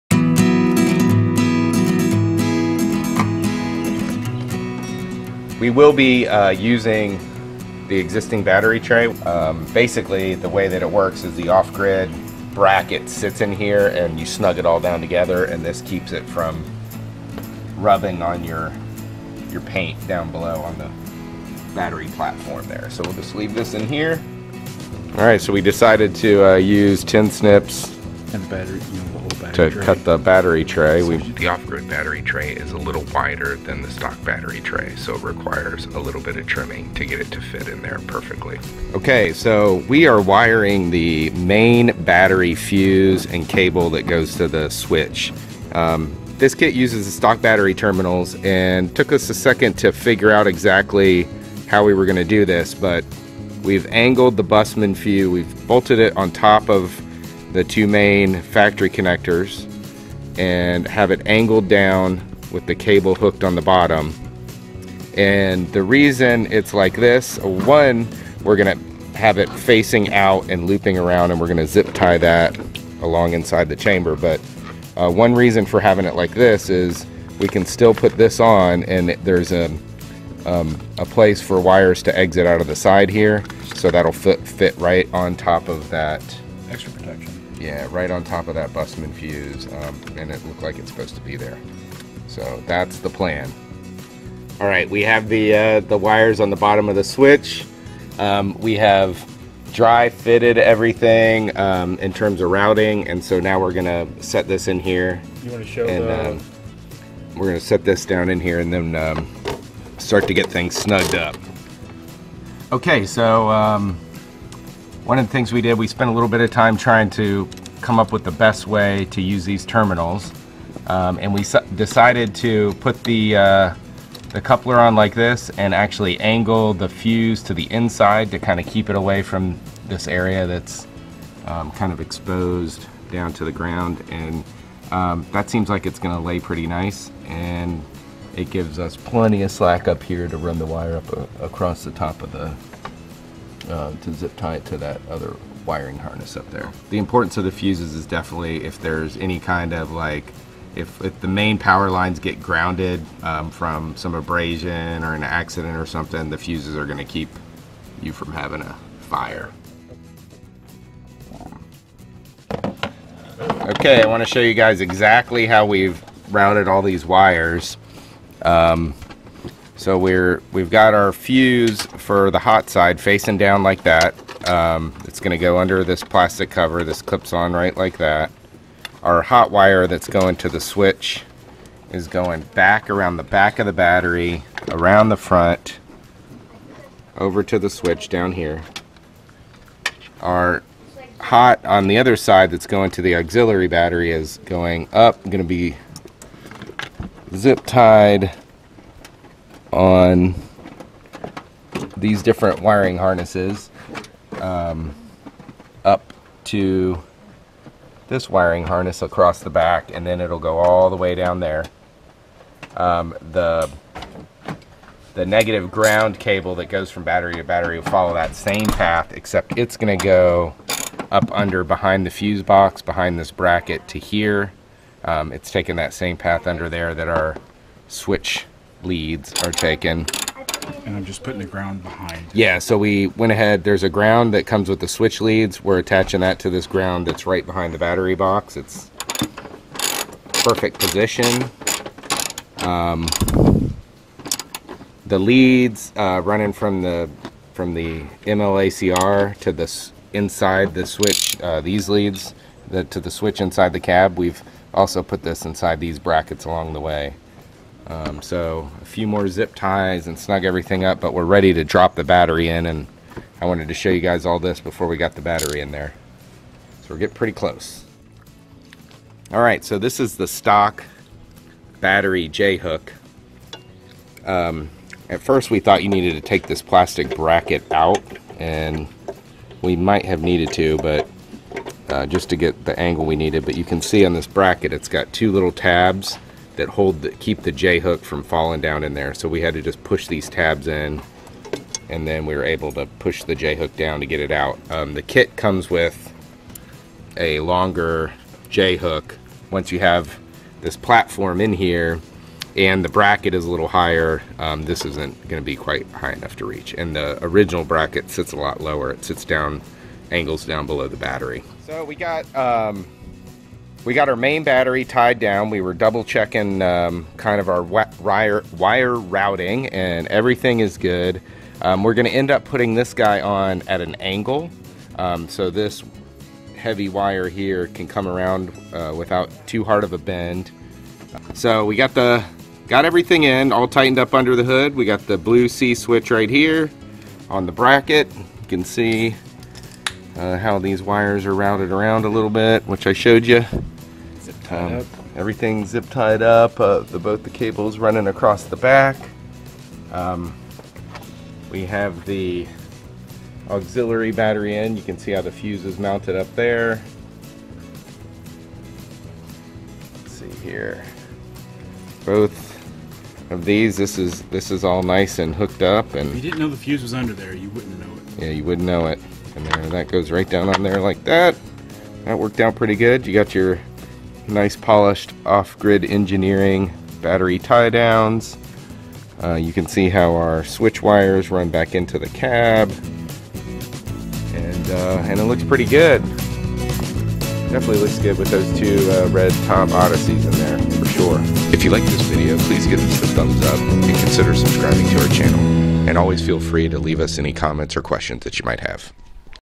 (4 of 5) 5th Gen 4Runner Dual Battery - Start Battery Tray - Wiring Install
In part 4 we install the start battery tray and wire everything up.

We finally got the Off-Grid Engineering 5th Gen 4Runner Dual Battery Install complete.  It took a little longer than we expected since we were careful to make sure we were setup for long haul overlanding expeditions off grid with no failure.  We installed two PC1400 odyssey batteries and they look great under the hood.  The blue sea ML-ACR is impressive and works flawlessly.  One of the first things any overland 4x4 vehicle needs is an isolated power system to support accessories, 12 volt fridge, lighting, chargers, etc.  Keep an eye out for the rest of the series we will be posting in the next week or so.  These also fit toyota tacomas.  This one also shows how we hook up the bussman or buss fuses.